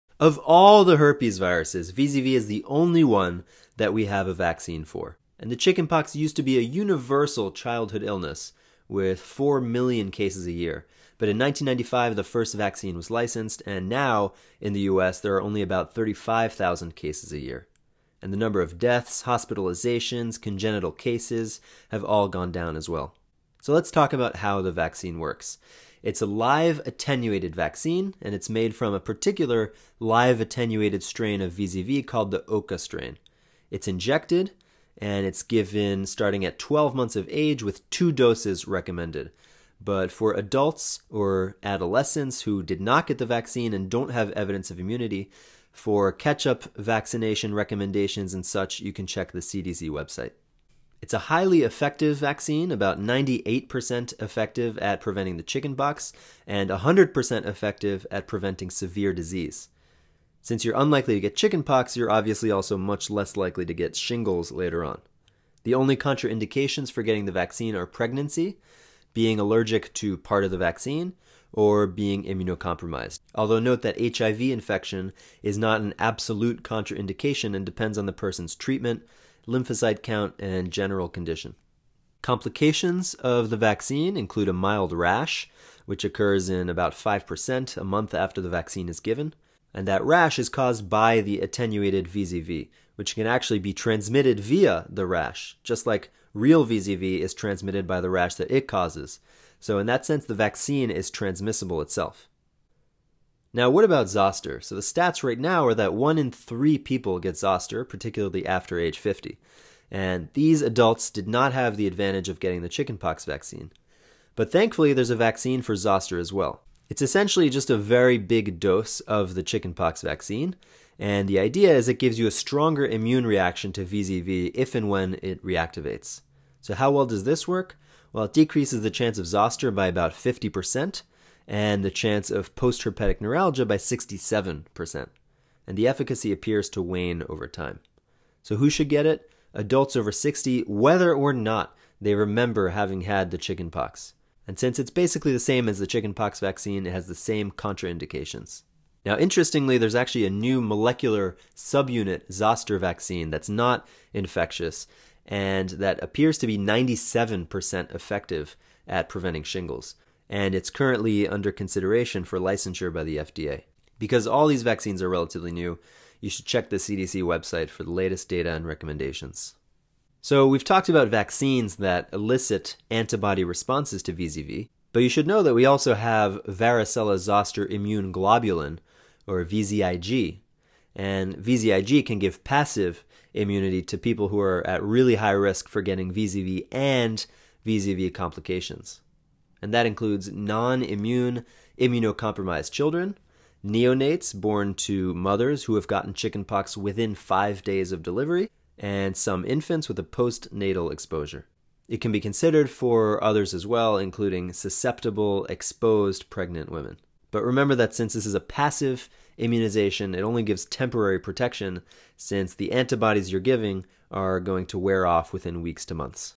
Chickenpox and Shingles (Zoster) Prevention
The VZV (Chickenpox) vaccine has reduced infections from 4 million to 35 thousand infections per year. Learn more about the vaccine and how we treat chickenpox and zoster in this video.